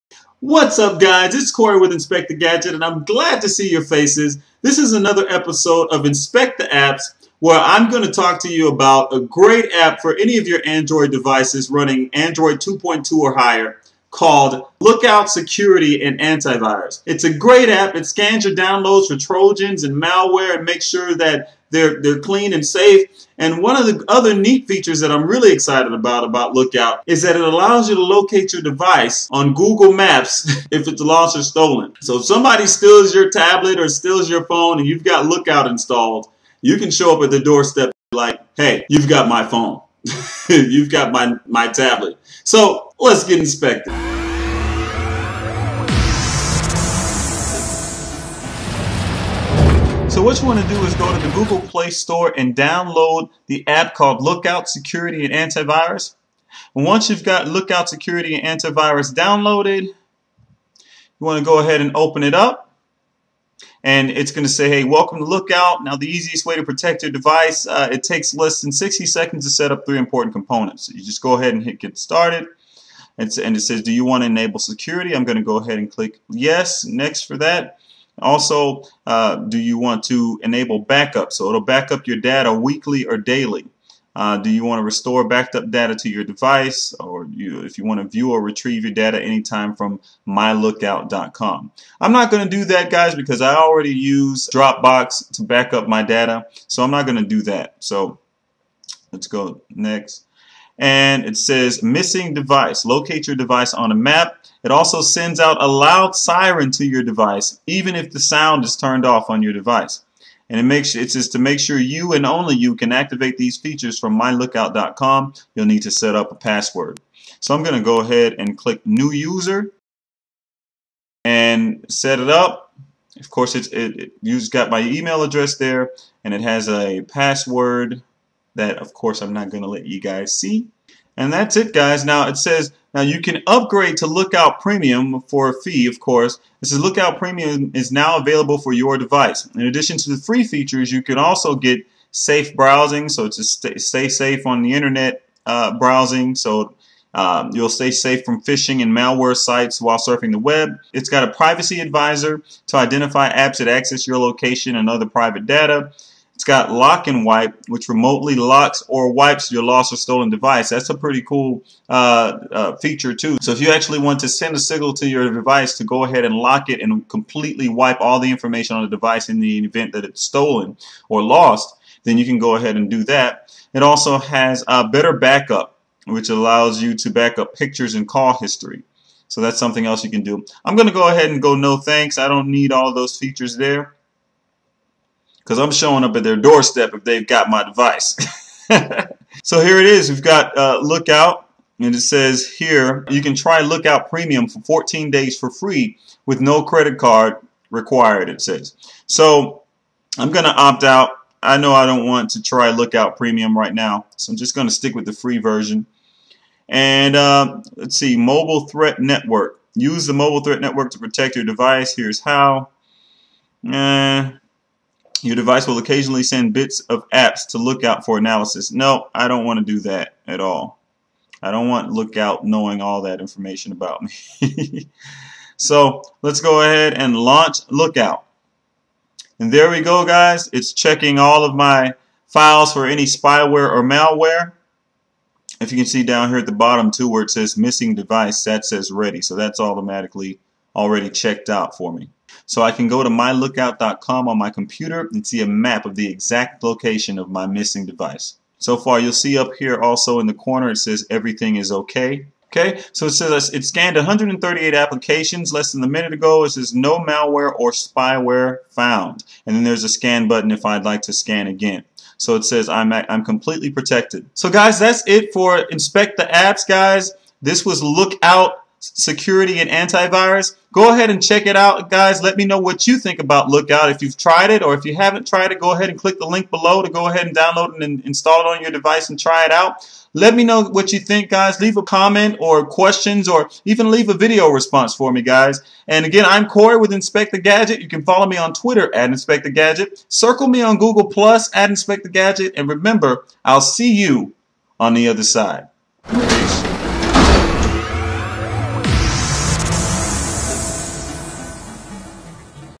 APPS: LOOKOUT ANDROID LOCATOR
Korre with InspecThaGadget does another episode of InspecThaApps with an app called Lookout Security and Antivirus that uses Google Maps to locate your lost or stolen Android device.